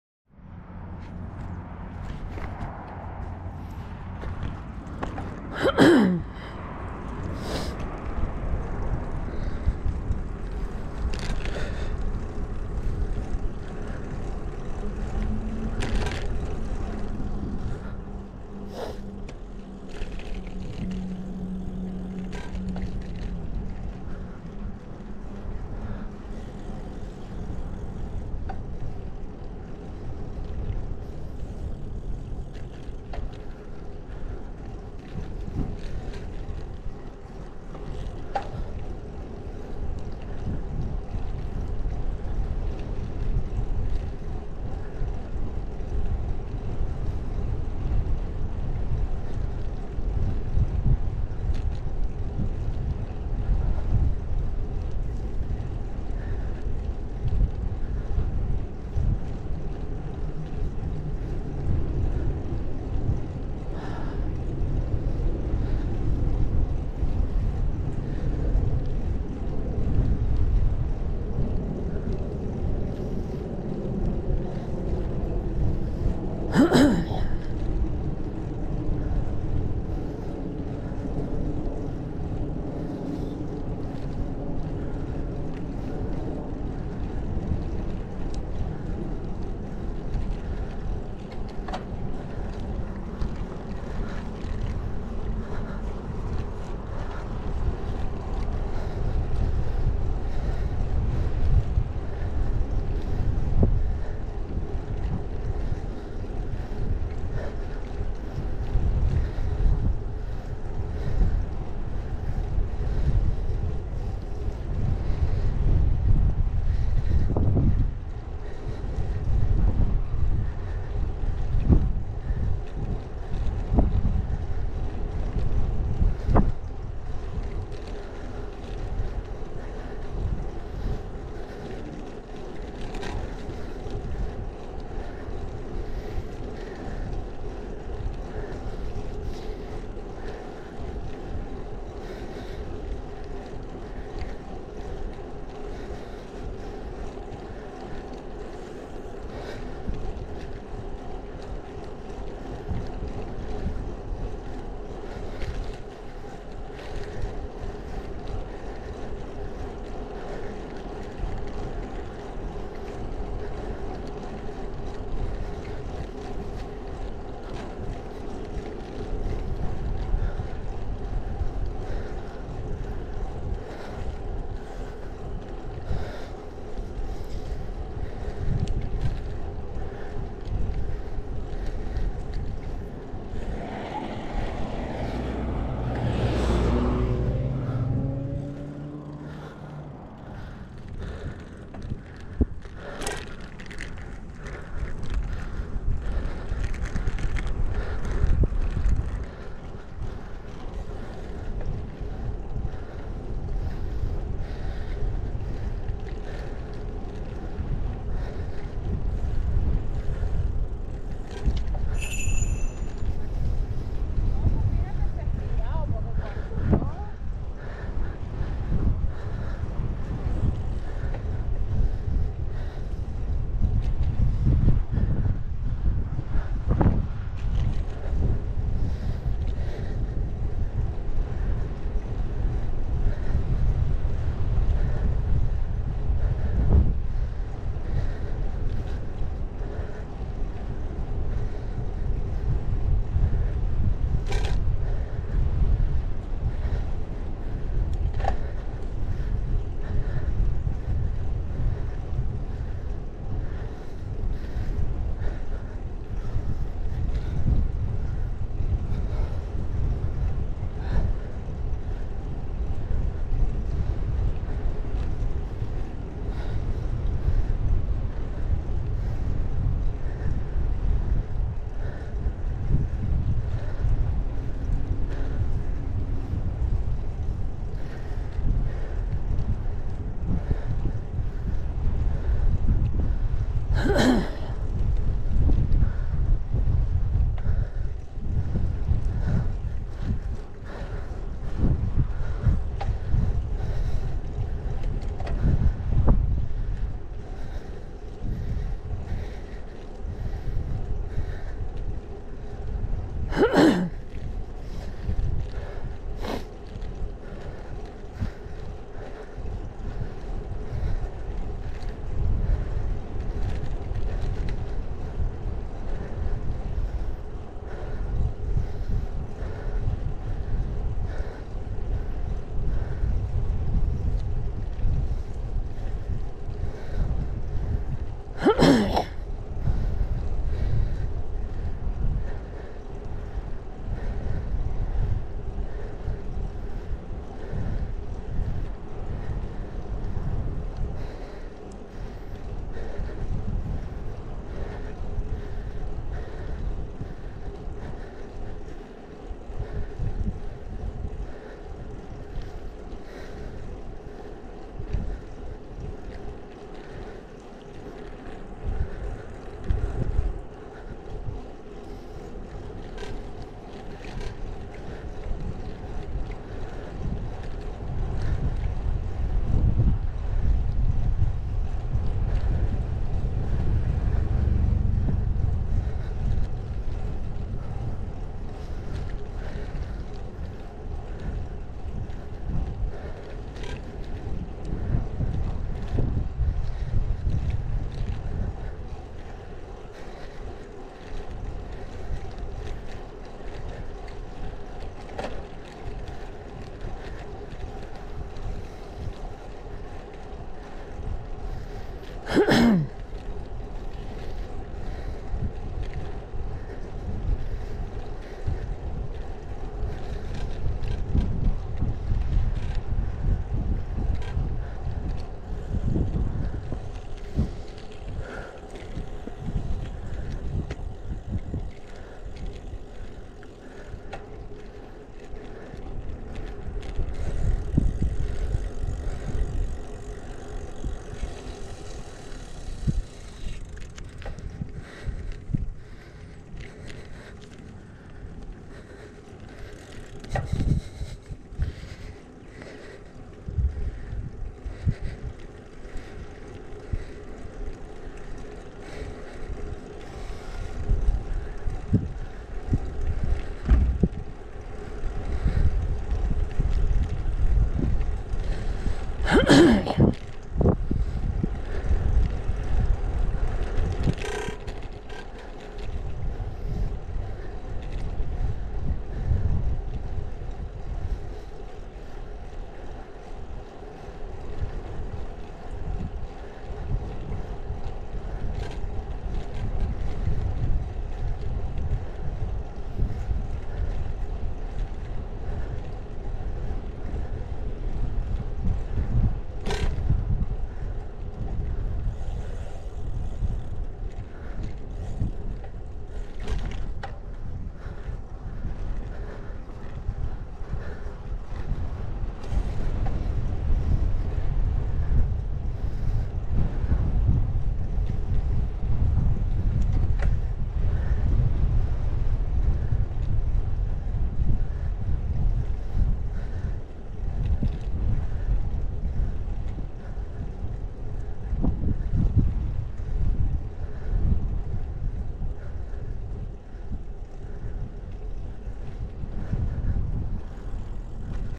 ALMOST GOT KILLED!!! POINCIANA, FL PART 2 (Stepping Stone pt. 1) (ZIP CODE 34758)
ABOUT 3 MINUTES INTO THE RIDE, THIS MANIAC MAKES A RIGHT TURN INTO A RESIDENTIAL STREET BY A STOP SIGN INTERSECTION AT LIKE 40 MPH AND DIDN'T EVEN SLOW DOWN OR APOLOGIZE!!! Community in Poinciana.